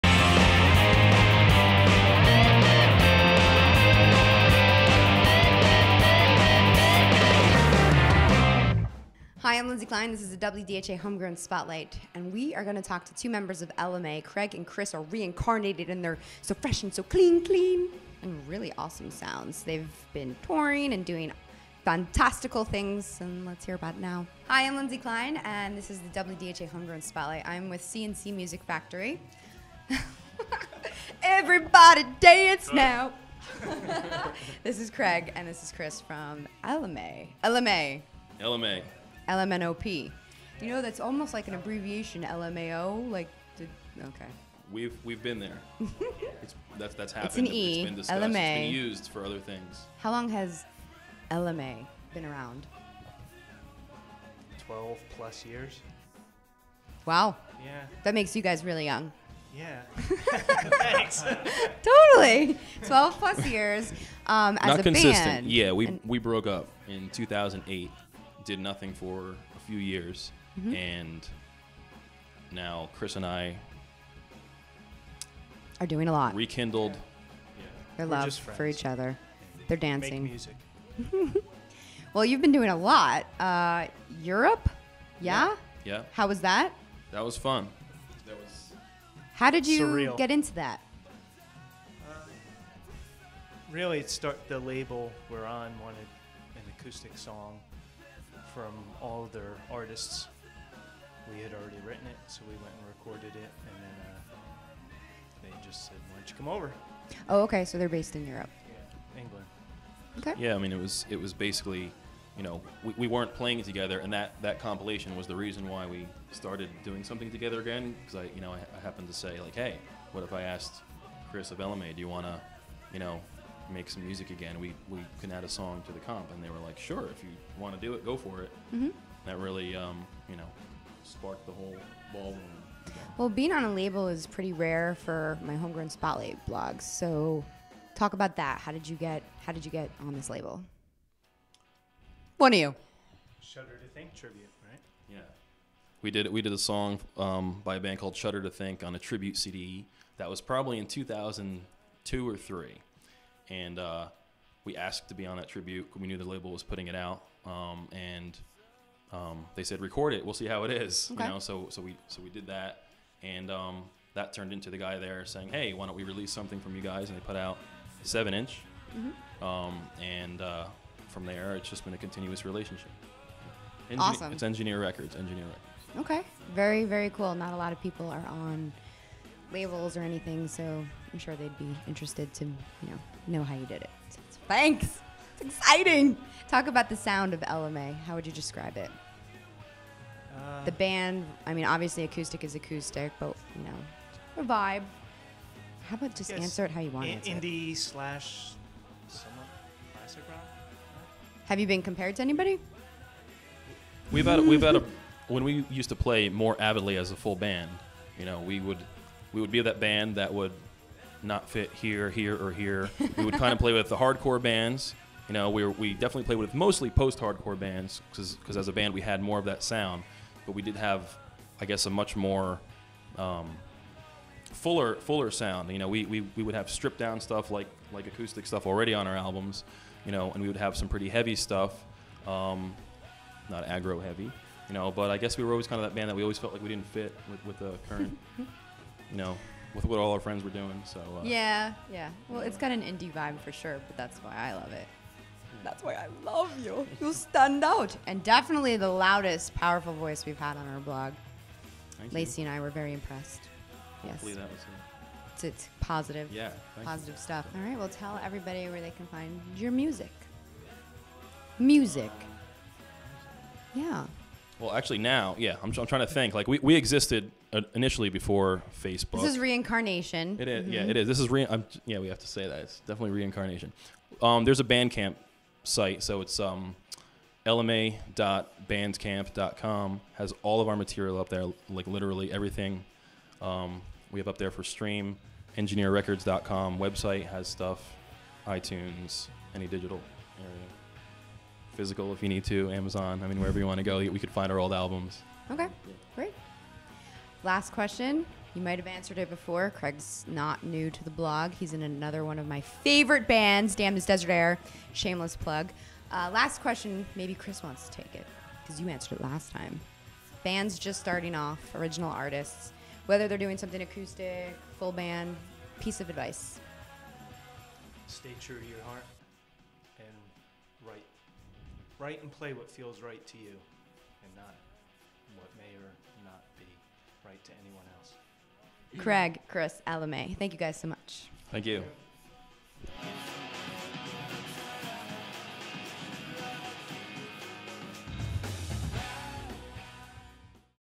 WDHA's Home Grown Spotlight: Elemae
WDHA's Lindsay Klein interviews two members of the band, Elemae, during this week's Home Grown Spotlight.